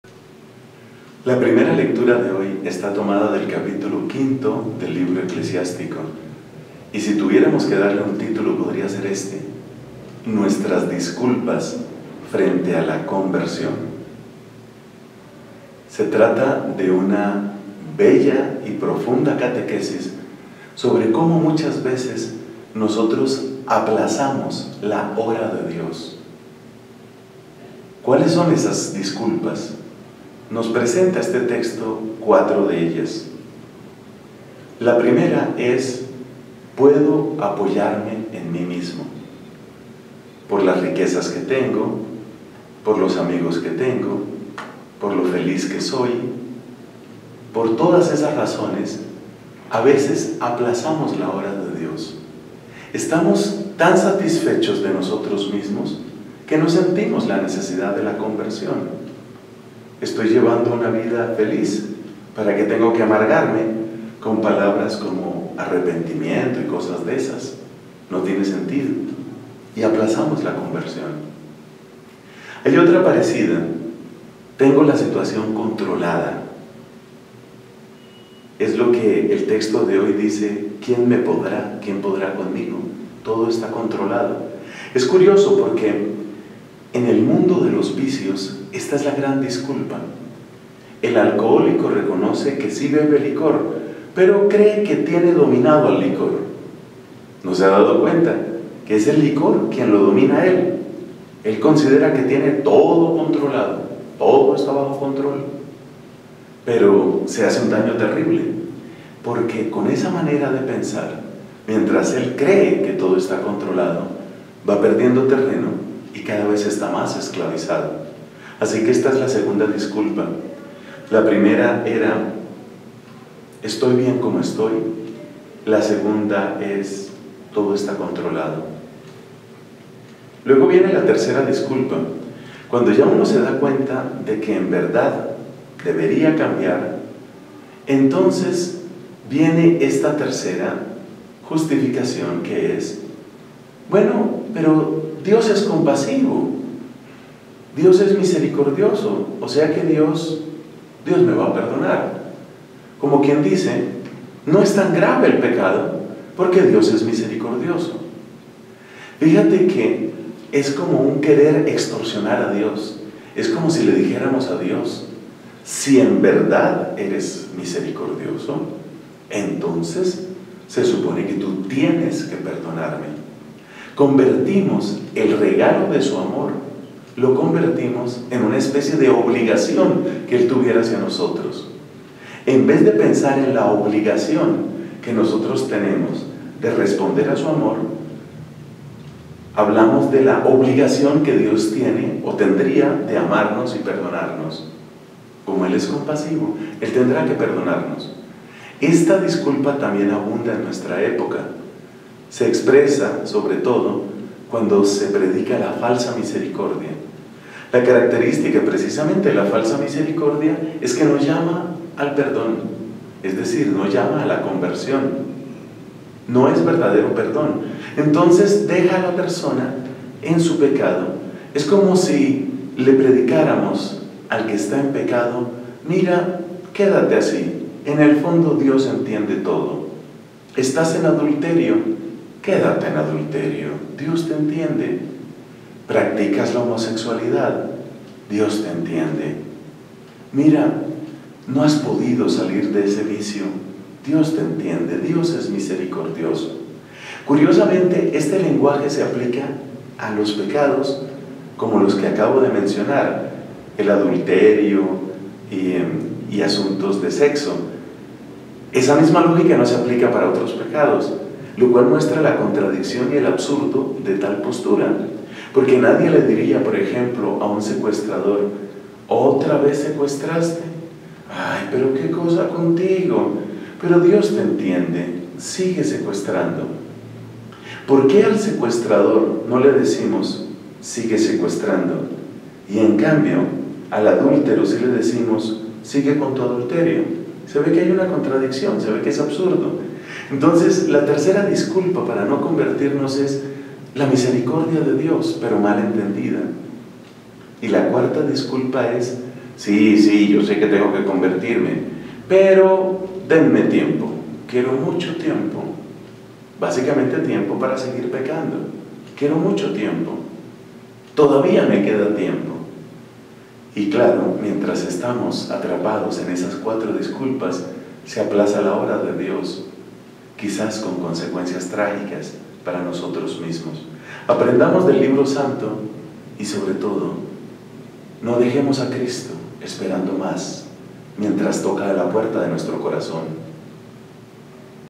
LA GRACIA del Jueves 28 de Febrero de 2019
No dejemos a Cristo esperando más mientras toca a la puerta de nuestro corazón, no aplacemos su obra en nosotros buscando disculpas para no decidirnos por la conversión.